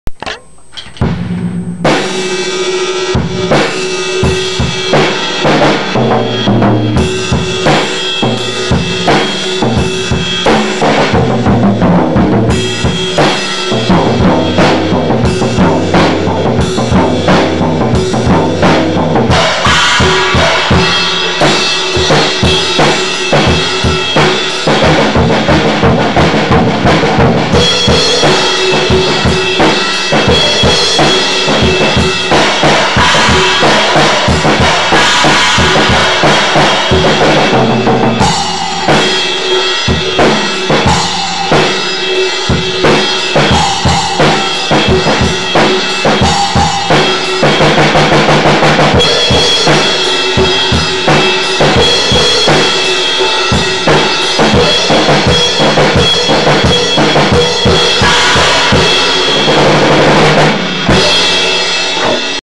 Me Warming up on my mapex drum set
just got some new evans hydrolic drum heads